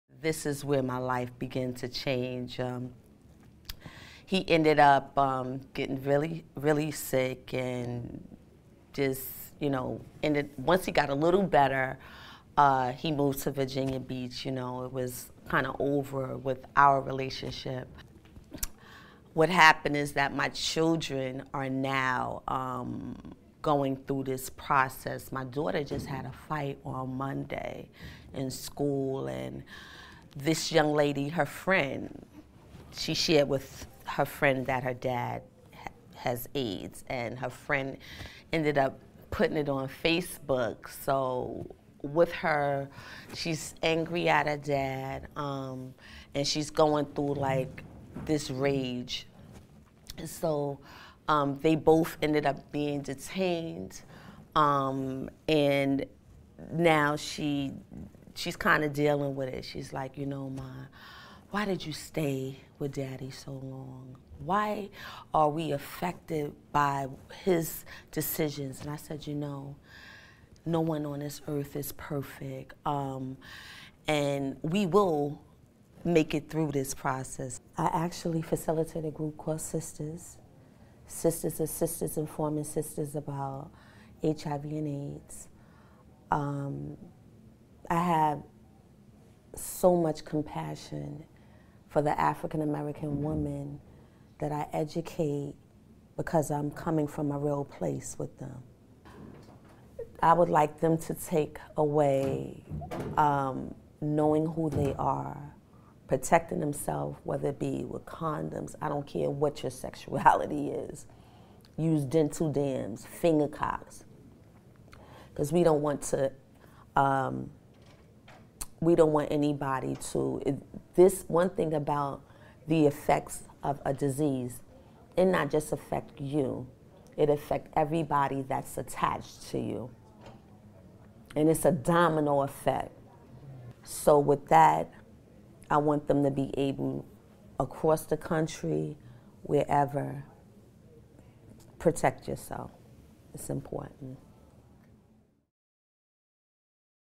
Nicole, Yonkers, NY
Nicole is a health educator at Sharing Community and facilitates a group called Sisters, which teaches and empowers women infected and/or affected by HIV/AIDS.  She raises her 3 children on her own after she separated from her ex-husband when he revealed that he was HIV+.